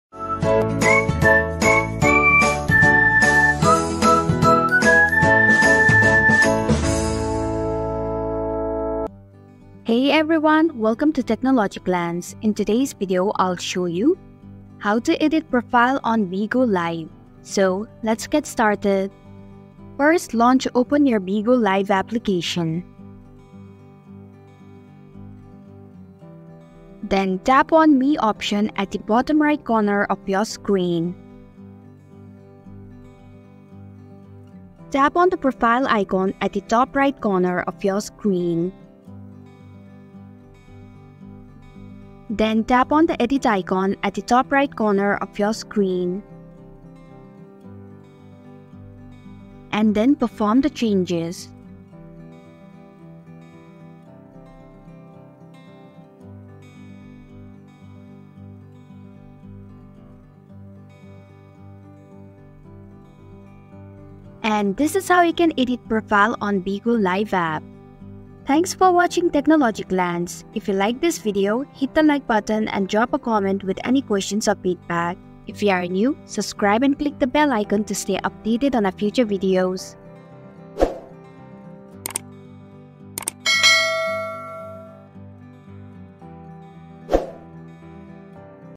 How to Edit Profile on Bigo Live? | Technology Glance
Curate your Bigo Live presence! This tutorial guides you through the steps to edit your profile on Bigo Live. Whether it's updating your bio, changing your profile picture, or adding a personal touch, learn how to make your profile uniquely yours. Watch and showcase the best version of yourself on Bigo Live.